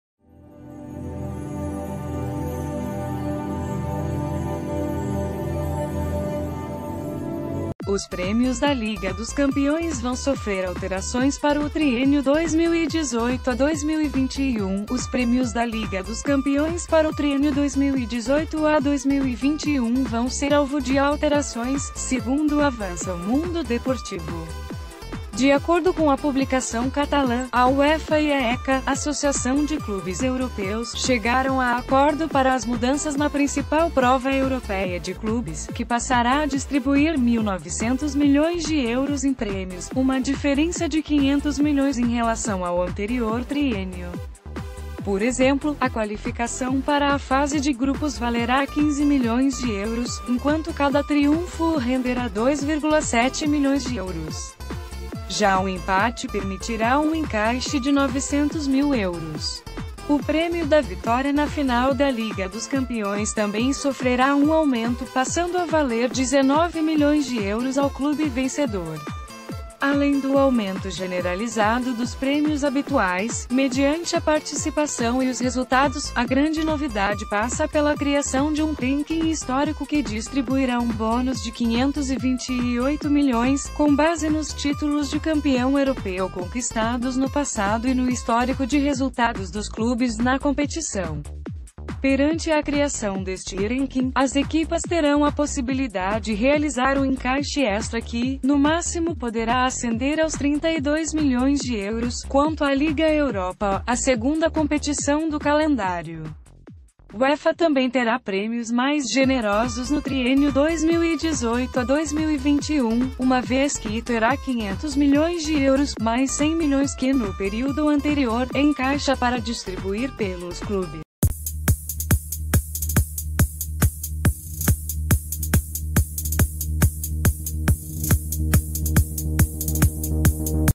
Últimas Notícias | Champions: prémios mais generosos, ranking histórico e aumento na Liga Europa
Os prémios da Liga dos Campeões vão sofrer alterações para o triénio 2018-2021.Os prémios da Liga dos Campeões para o triénio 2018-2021 vão ser alvo de alterações, segundo avança o Mundo Deportivo. De acordo com a publicação catalã, a UEFA e a ECA (Associação de Clubes Europeus) chegaram a acordo para as mudanças na principal prova europeia de clubes, que passará a distribuir 1900 milhões de euros em prémios, uma diferença de 500 milhões em relação ao anterior triénio. Por exemplo, a qualificaçã...